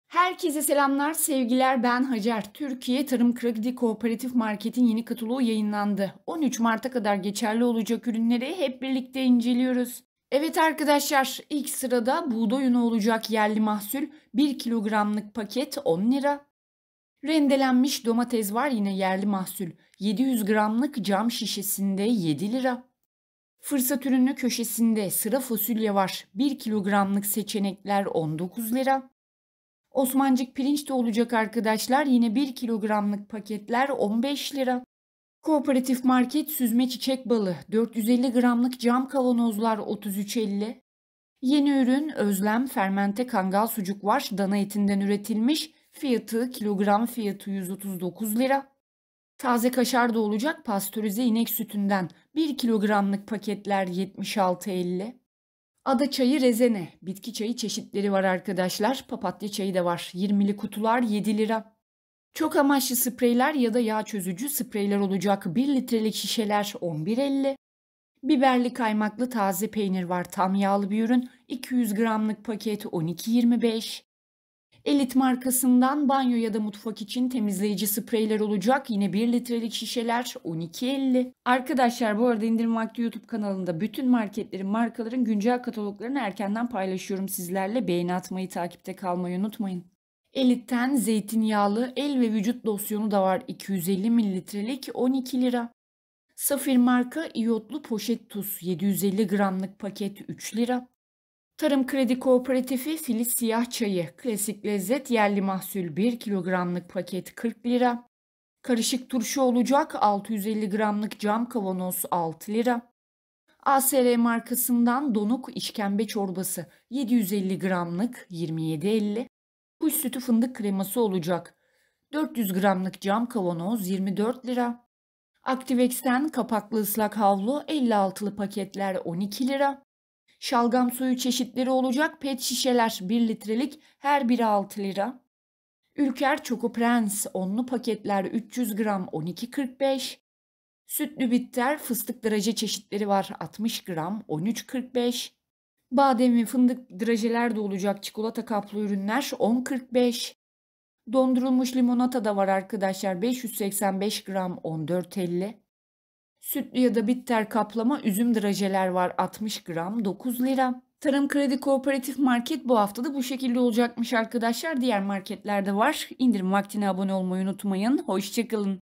TARIM KREDİ MARKET İNDİRİMLERİ | KOOPERATİF MARKET KATALOĞU | TARIM KREDİ KOOPERATİFLERİ MARKET
Tarım kredi kooperatif market kataloğu yeni hafta fırsat ürünleri belli oldu. Tarım kredi market son günü 13 mart 2022 kataloğu tek tek inceliyoruz.

Elit Çok amaçlı Sprey Temizleyici 1000 ml
Çokoprens
Activex Islak Havlu
dondurulmuş İşkembe Çorbası
Kuş Sütü Fındık Ezmesi
Tarım Kredi Filiz Çay 1 Kg
Sıra Fasulye 1 kg
Tarım Kredi Pirinç 1 kg
Rendelenmiş Domates 700 g
Yerli Mahsül Buğday Unu 1 kg
Zeytinyağlı Sıvı Sabun